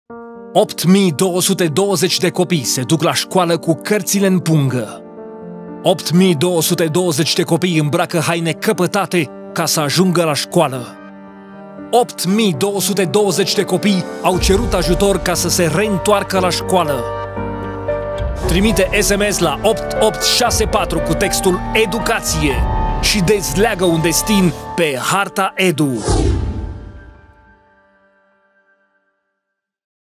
1 copil încălțat = 1 destin dezlegat
Pentru 8220 de copii, lipsa
pantofilor și a rechizitelor blochează drumul spre
educație. Noi credem în puterea și sprijinul comunității.

Cu doar 4 euro pe lună, trimiți un copil
👉Trimite un SMS cu textul 'EDUCATIE' la 8864 și fii
parte din schimbare.